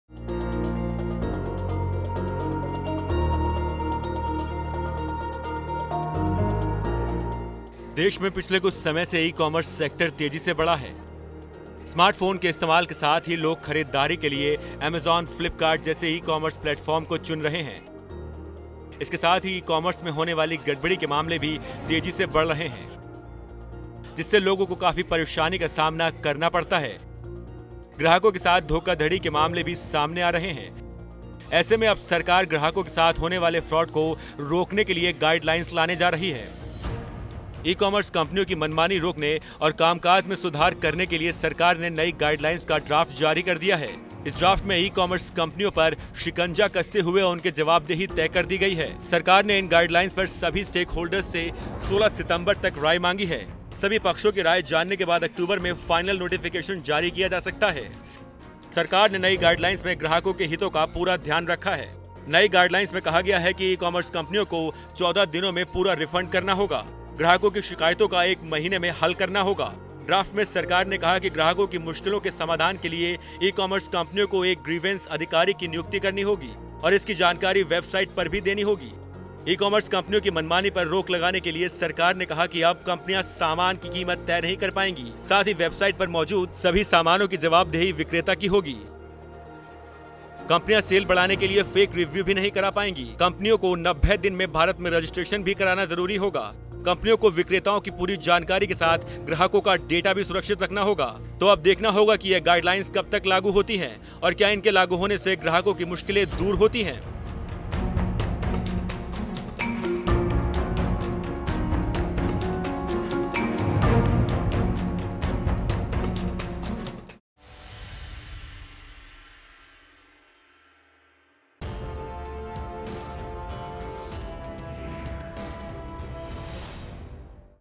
Amazon, Flipkart की मनमानी अब नहीं चलेगी!| Biz Tak
To protect consumers’ interest, the Centre has proposed guidelines for e-commerce firms that entail a 14-day deadline to effect refund request, mandate e-tailers to display details of sellers supplying goods and services on their websites and moot the procedure to resolve consumer complaints.